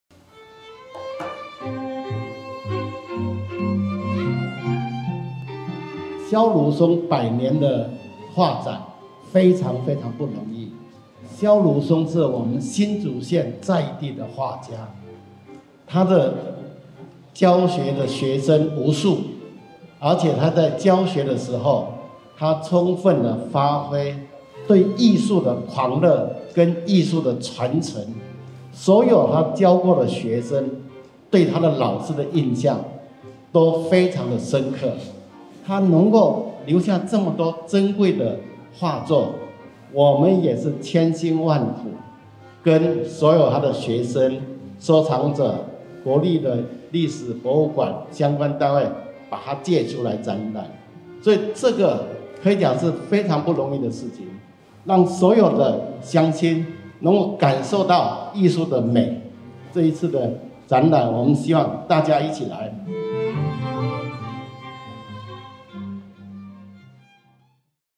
蕭如松紀念展今開幕 楊文科邀鄉親浸淫美學世界
客籍藝術家蕭如松今年適逢百歲冥誕，新竹縣政府盛大舉辦系列活動，其中「松色經行──2022蕭如松百歲冥誕紀念巡迴展」今（3日）開幕，新竹縣長楊文科與多名藝文界人士均到場，邀請鄉親一同到新竹縣文化局美術館參展，浸淫在蕭如松的美學世界當中。